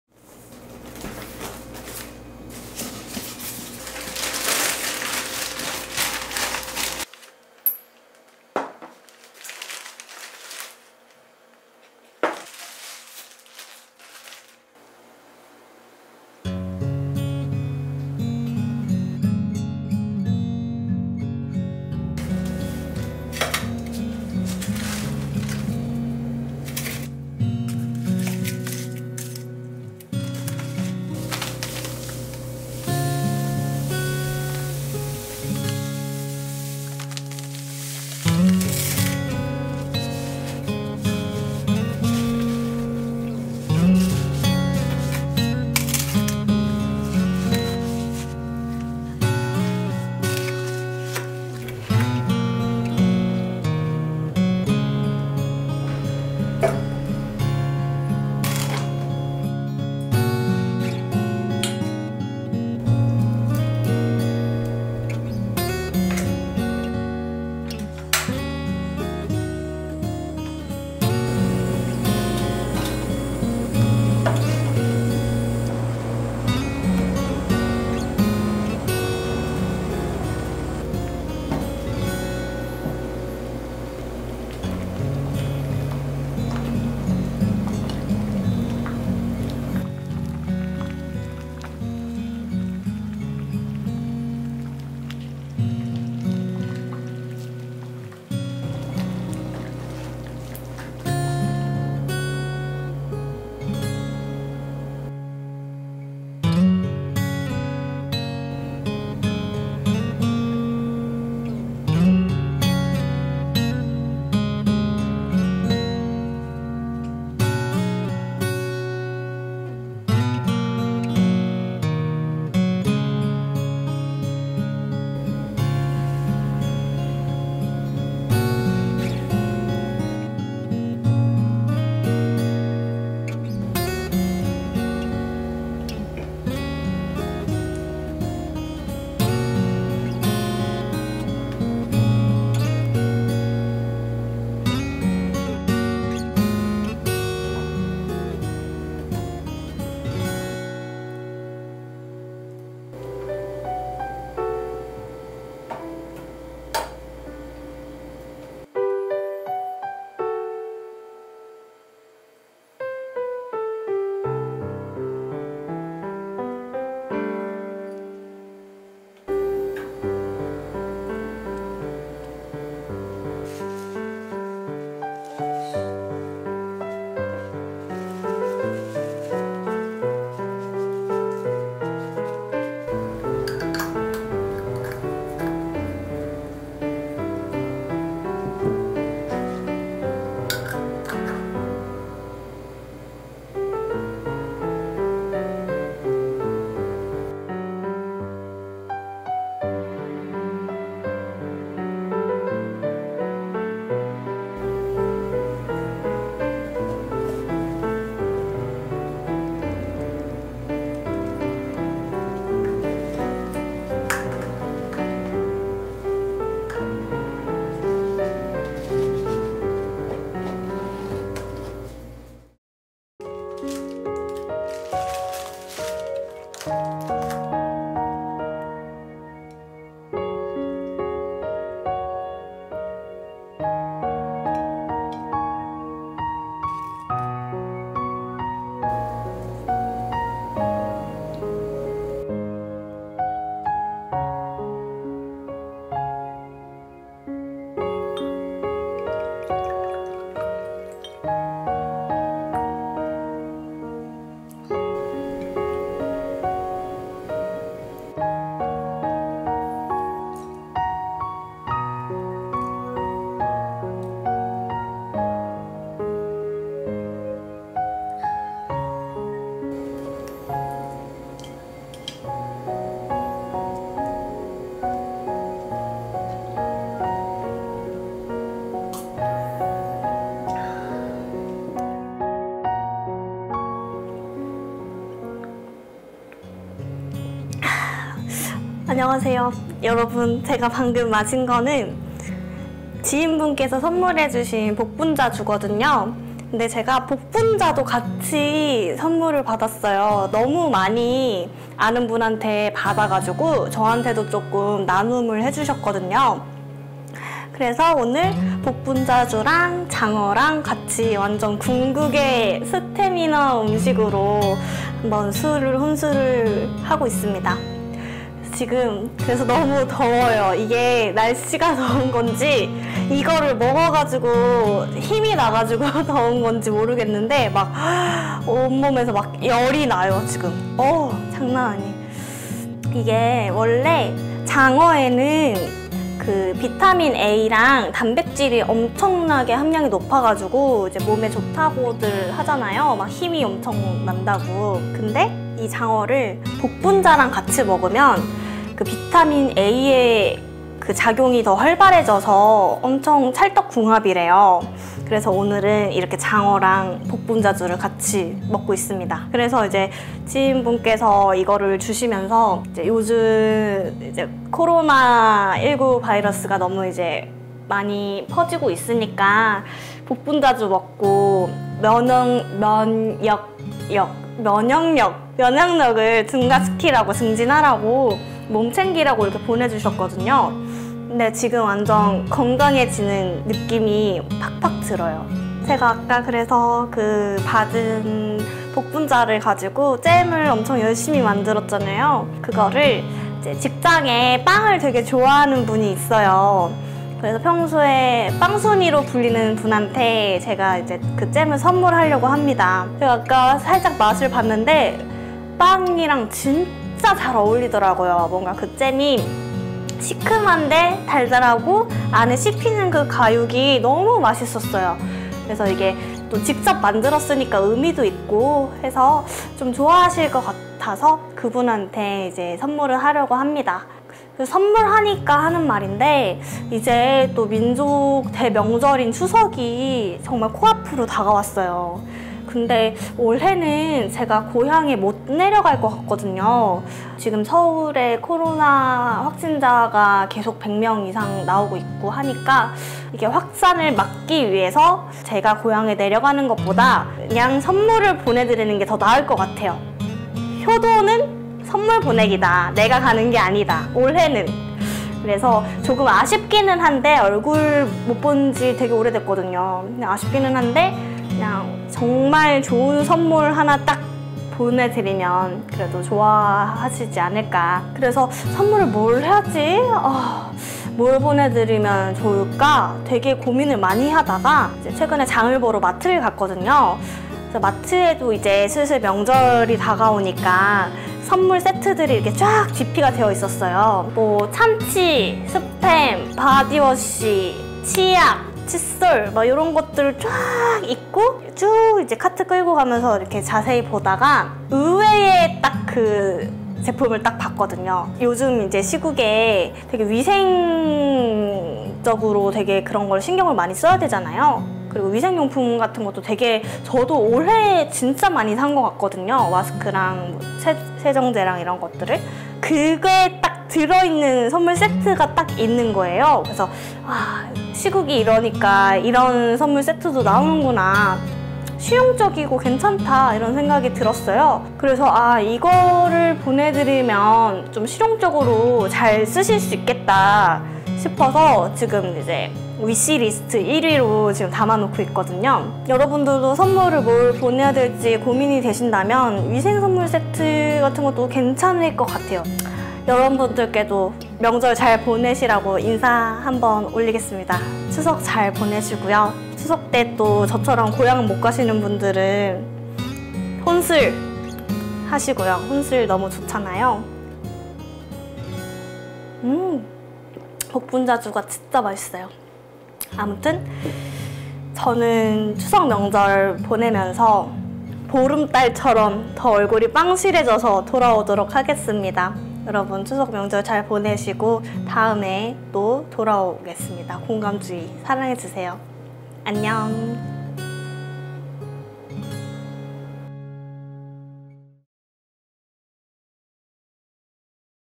[공감주희] 올 추석, 진짜 '선물'의 의미ㅣ추석선물추천ㅣ추석ㅣ복분자잼ㅣ복분자주ㅣ장어
"올 추석 만큼은 내가 가는게 아니라
'선물'을 보내는게 더 효도다"

곧 추석인데요, 직장, 학업 등의 이유로 멀리 떨어져 지내서
오랫동안 가족분들을 뵙지 못한 분들이 정말 많을 거에요.

너무너무 아쉽지만 그래도 이번 명절은
멀리서 마음을 전달해야하는게 더 맞겠죠?!

여러분들은 이번 추석에 아끼는 분들을 위해
어떤 선물을 하실 예정인가요?!

혼자가 편하지만 외롭기도 한 오늘
"오늘 하루도 수고 많으셨습니다"

여러분의 많은 사연을 기다립니다.